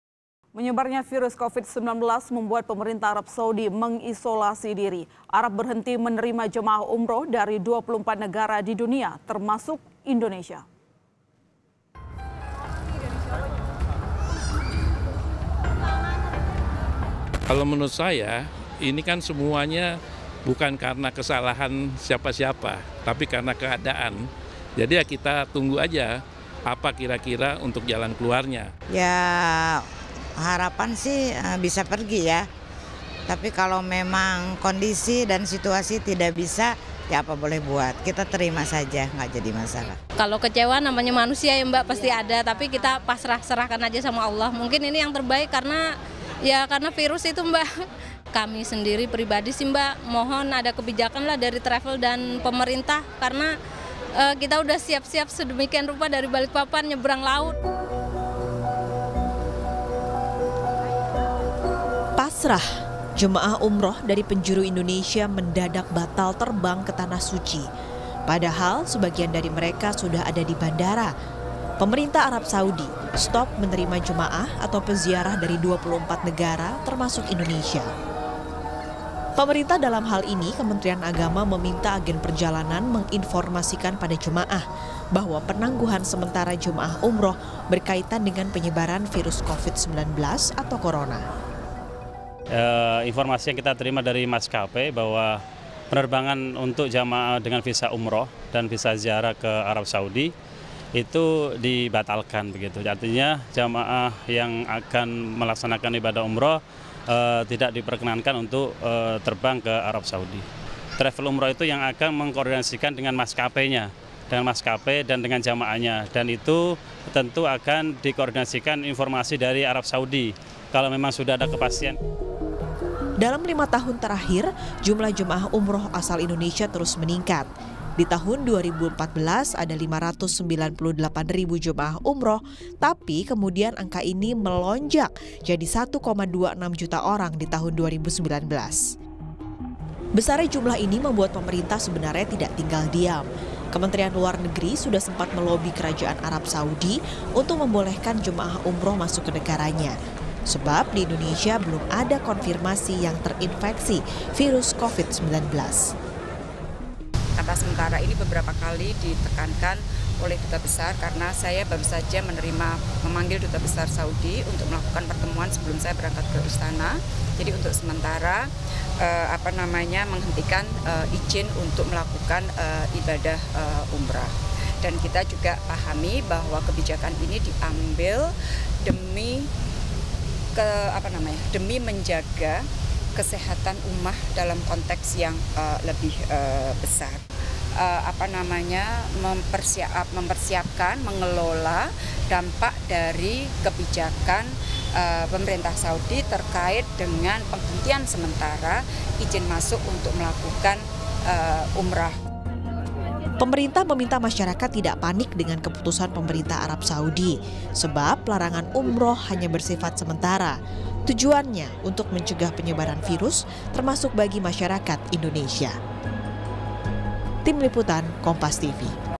Arab Saudi Hentikan Umrah, Pemerintah Minta Masyarakat Tidak Panik
JAKARTA, KOMPAS.TV - Penyebaran virus covid-19 membuat pemerintah Arab Saudi mengisolasi diri. Arab berhenti menerima jemaah umrah dari 24 negara di dunia, termasuk Indonesia. Pasrah, jemaah umrah dari penjuru Indonesia mendadak batal terbang ke tanah suci. Padahal, sebagian dari mereka sudah ada di bandara. Pemerintah Arab Saudi, stop menerima jemaah atau peziarah dari 24 negara, termasuk Indonesia. Pemerintah, dalam hal ini Kementerian Agama meminta agen perjalanan menginformasikan pada jemaah. Bahwa penangguhan sementara jemaah umrah, berkaitan dengan penyebaran virus covid-19 atau Corona. Dalam 5 tahun terakhir, jumlah jemaah umrah asal Indonesia terus meningkat. Di tahun 2014, ada 598 ribu jemaah umrah, tapi kemudian angka ini melonjak jadi 1,26 juta orang di tahun 2019. Besarnya jumlah ini membuat pemerintah sebenarnya tidak tinggal diam. Kementerian Luar Negeri sudah sempat melobi Kerajaan Arab Saudi, untuk membolehkan jemaah umrah masuk ke negaranya. Sebab di Indonesia belum ada konfirmasi yang terinfeksi virus covid-19 atau corona. Pemerintah meminta masyarakat tidak panik dengan keputusan pemerintah Arab Saudi. Sebab, larangan umrah hanya bersifat sementara. Tujuannya untuk mencegah penyebaran virus, termasuk bagi masyarakat Indonesia.